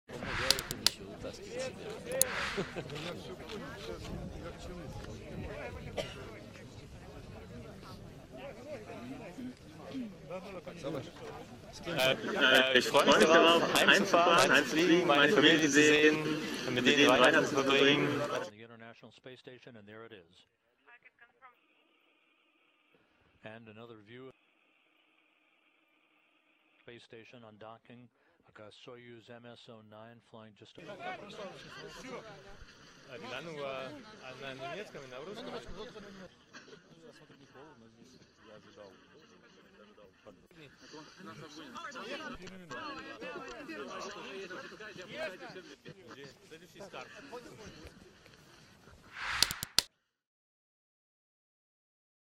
Nach ISS-Einsatz: Alexander Gerst ist wieder gelandet | DER SPIEGEL
Fast 200 Tage lang war Astronaut Alexander Gerst auf der ISS, jetzt ist er zurück: Um kurz nach sechs Uhr landete er in Kasachstan. Noch am Abend wird er in Deutschland erwartet.